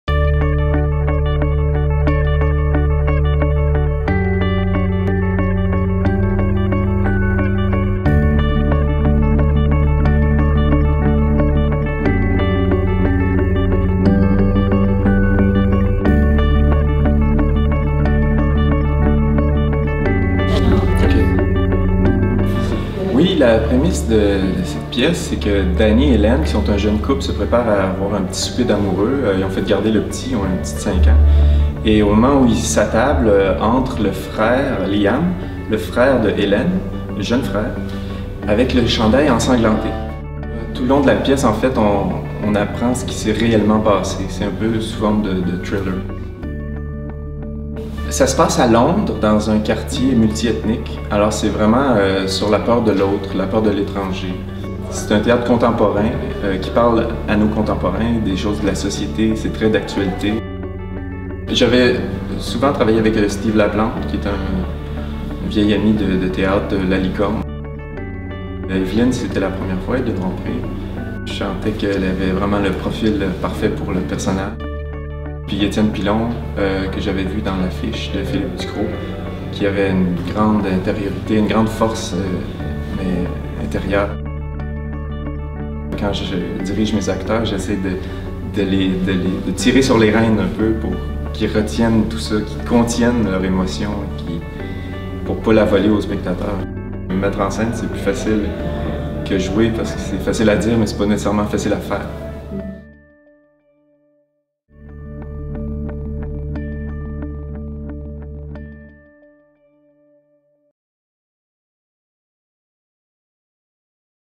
Orphelins | Théâtre de la Ville | 19 et 20 septembre 2013
Orphelins
Jeudi 19 et vendredi 20 septembre 2013
Salle Jean-Louis-Millette du Théâtre de la Ville

Devine qui vient dîner ? Un jeune homme au chandail couvert de sang ! Ce n'est pas nécessairement ce qu'on a en tête lorsqu'on pense à un souper romantique. C'est ainsi que Liam fait irruption dans la vie de sa sœur Helen et de son amoureux Danny en racontant qu'il a voulu secourir un ado et que les choses ont mal tourné. D'entrée de jeu, un climat de doute et de suspicion s'installe. Puis, la vérité se dévoile et ces trois écorchés révèlent leurs parts d'ombre et de mystère, leurs craintes et leurs valeurs, leur rapport trouble à la tolérance et aux étrangers.

Rencontre animée en compagnie de Jean-Denis Leduc le vendredi 20 septembre à 19 h 15.